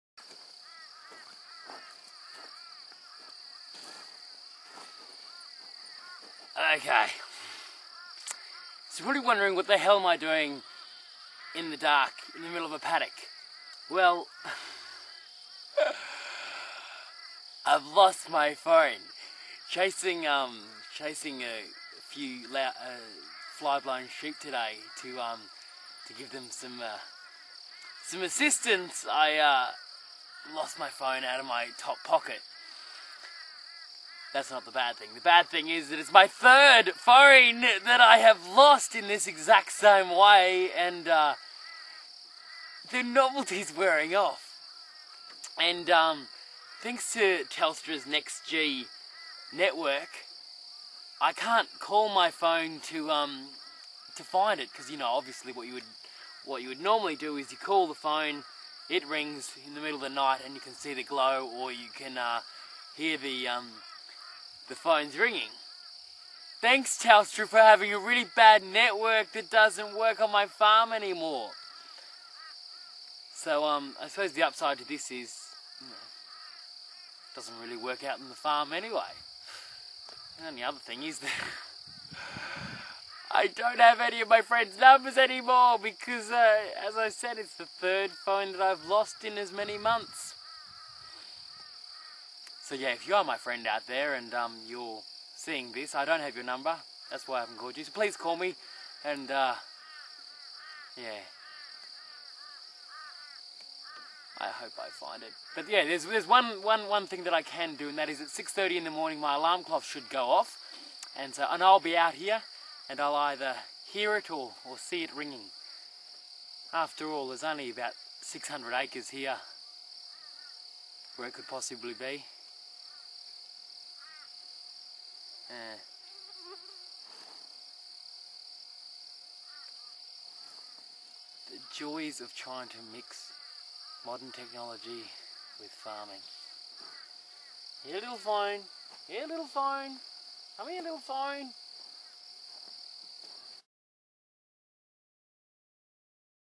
Lost Phone Saga (I'm always losing phones)
David Graham's Blog - Looking for lost phone in the dark